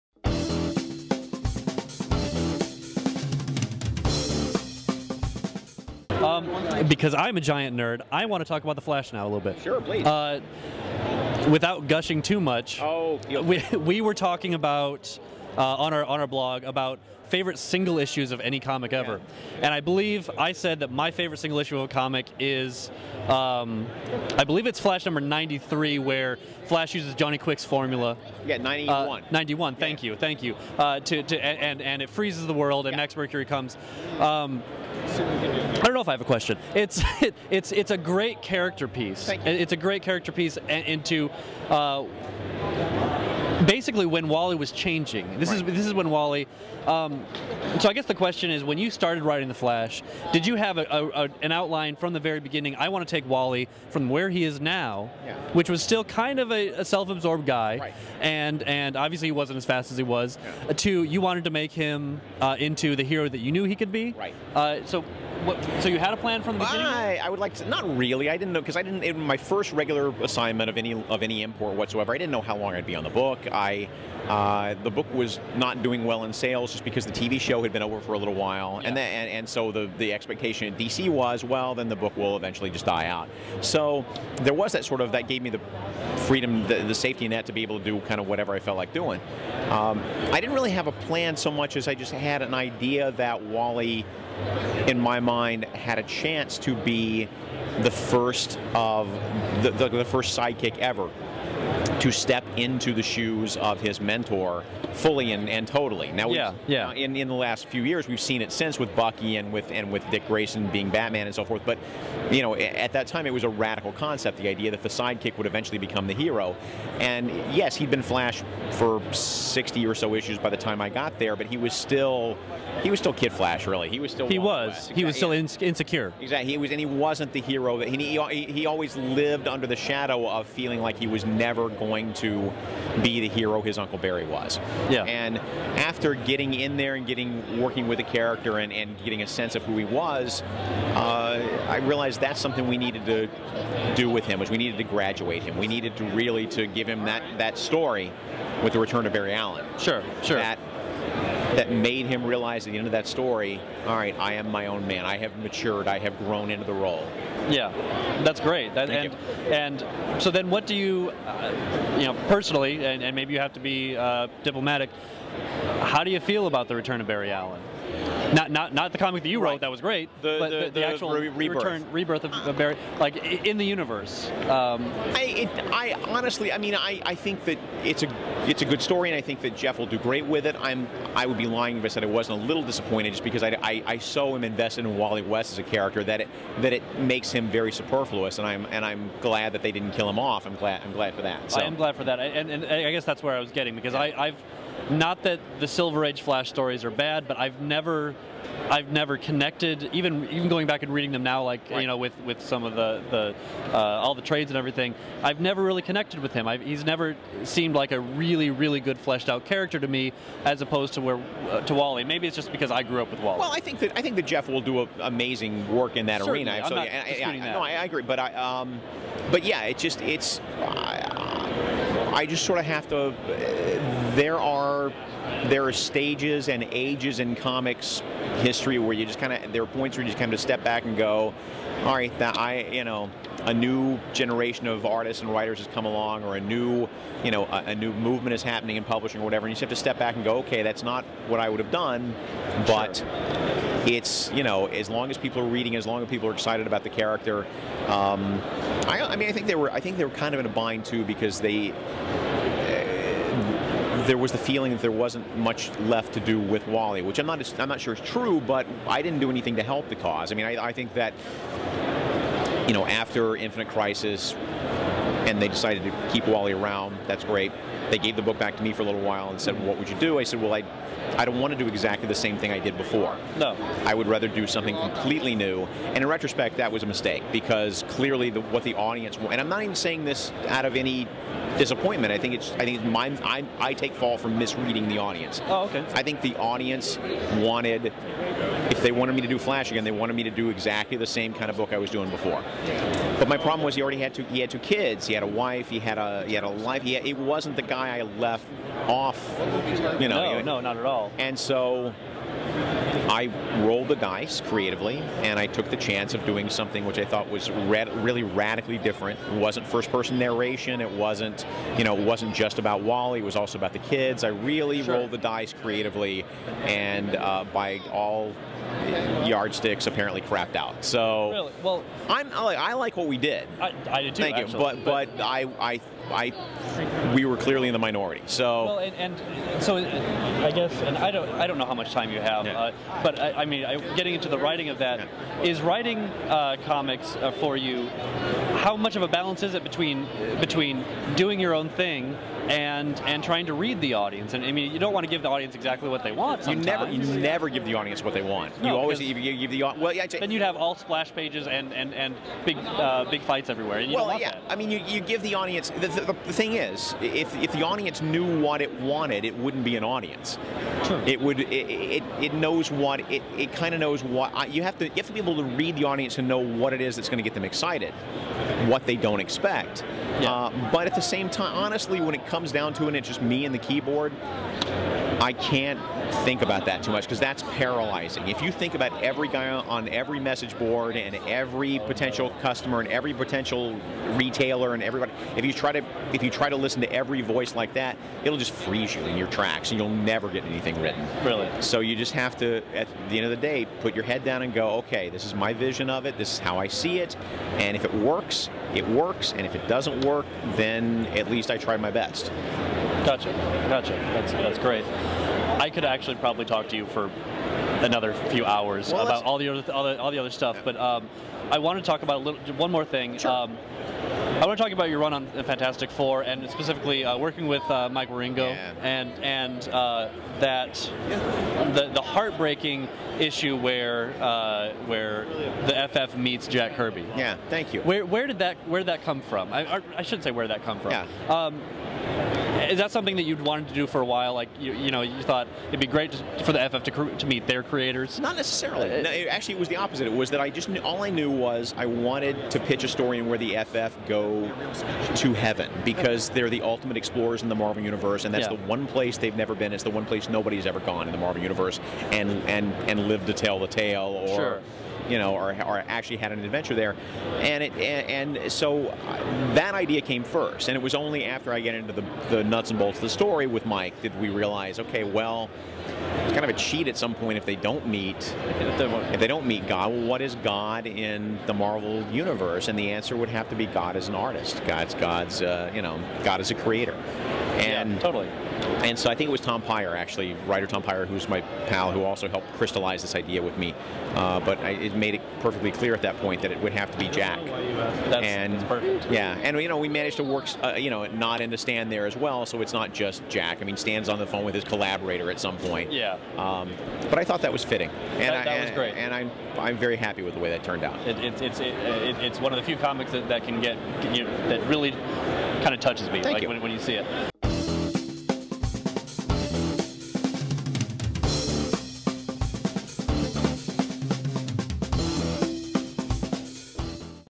Backroom Comics Podcast - Mark Waid Interview at ECCC 2010 Part 2
Part 2 of an interview with Mark Waid, creator of Irredeemable & Incorruptible, and Editor in Chief of Boom Studios, at the 2010 Emerald City ComiCon by the Backroom Comics Podcast.